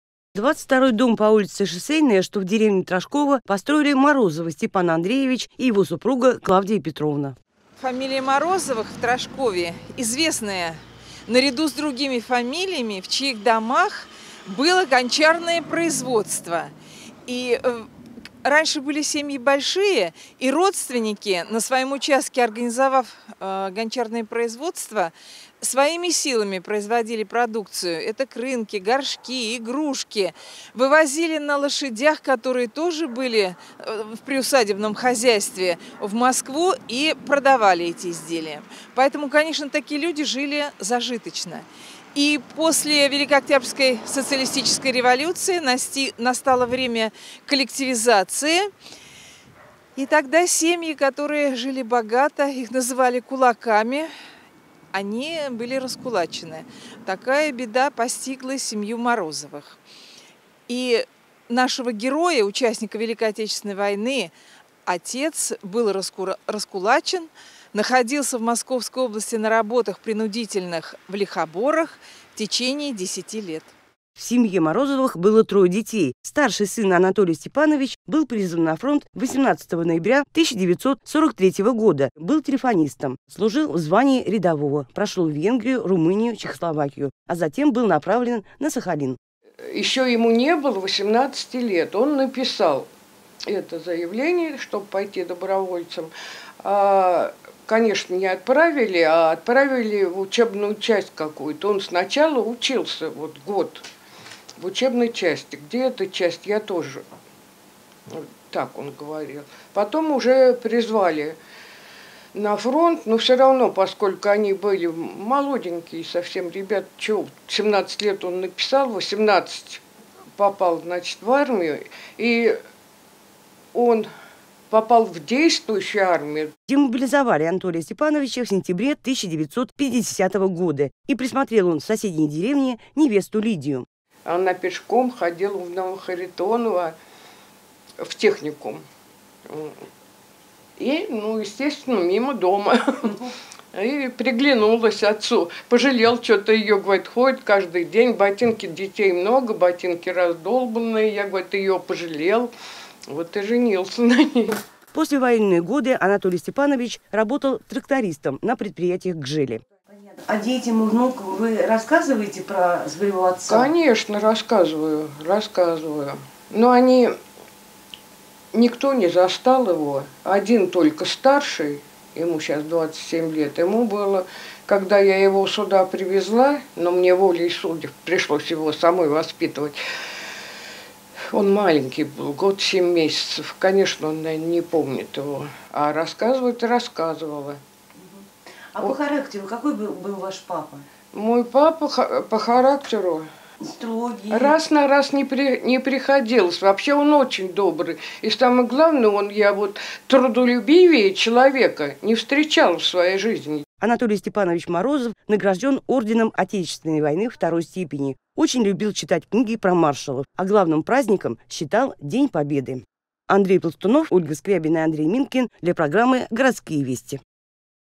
В Гжели продолжается акция «Дом Героя».
Морозов Анатолий Степанович жил в доме №22 в деревне Трошково. Был призван на фронт 18 ноября 1943 года. Был телефонистом в 16 артиллерийском полку. Воинское звание-рядовой. Прошел Венгрию, Румынию, а войну закончил в Чехословакии. Затем был направлен на Сахалин. Награжден орденом Отечественной войны второй степени. В Гжели продолжается акция «Дом Героя».